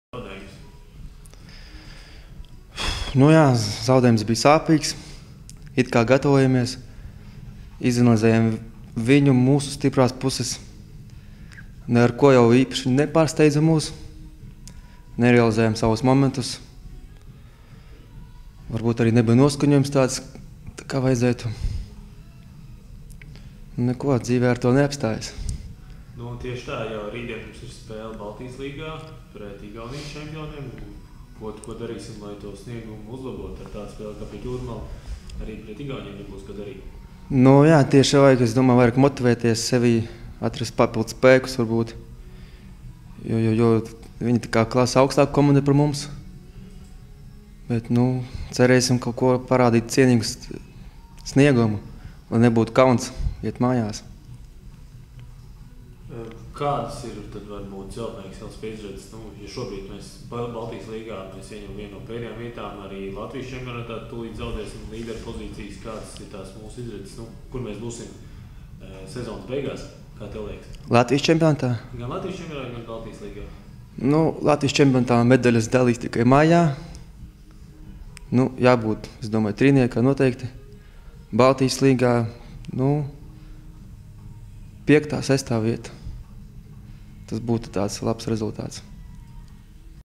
Žanis Malahovs 06.12.2013.
Celtnieks/LSPA handbolista Žaņa Malahova komentārs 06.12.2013. pēc zaudējuma LČ spēlē "Jūrmalas Sports", pirms BL spēles ar Igaunijas "Polva Serviti".